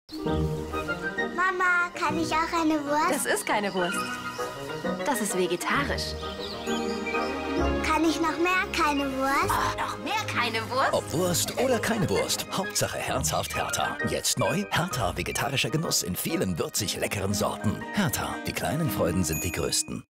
Herta - Ne Wurst
TV Spot for Herta as Digital Intermediate Technician and filmed on location in Cape Town, South Africa